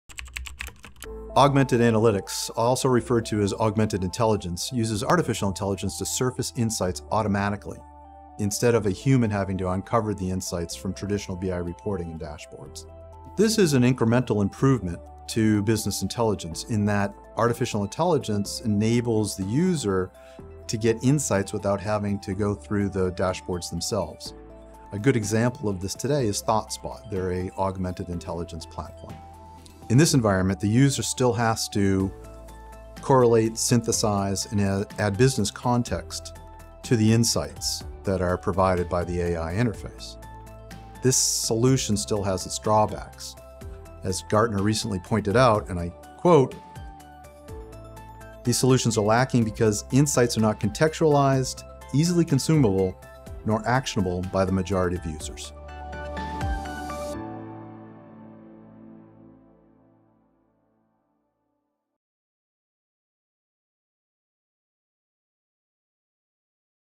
What is Augmented Intelligence?
Augmented Analytics, or Augmented Intelligence, uses AI to surface insights automatically instead of requiring a human to manually discover insights from traditional BI reports and dashboards. The main differences to Decision Intelligence?

• Insights are not contextualized
• They are not easily consumable
• Insights are not actionable